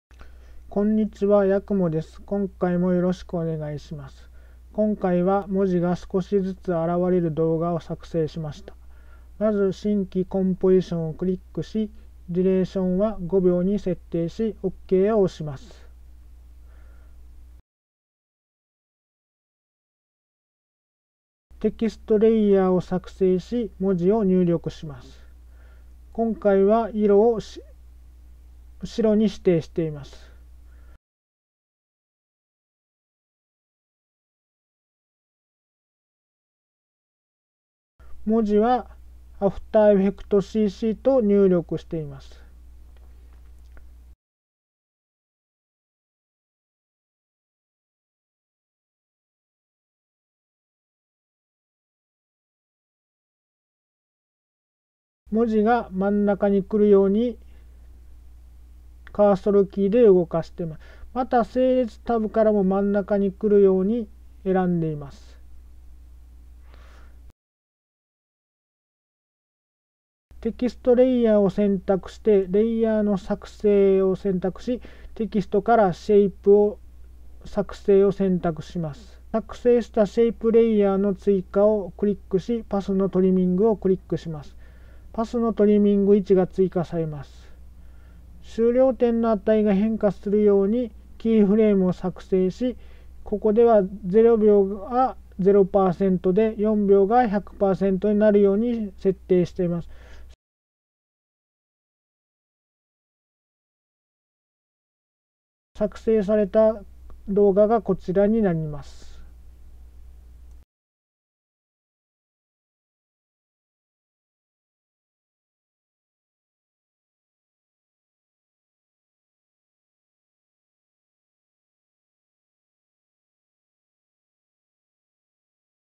動画編集テクニック講座 [After Effects] 文字が少しづつ現れる動画の作成方法 操作編
写真撮影、動画撮影、編集のご依頼をお待ちしています。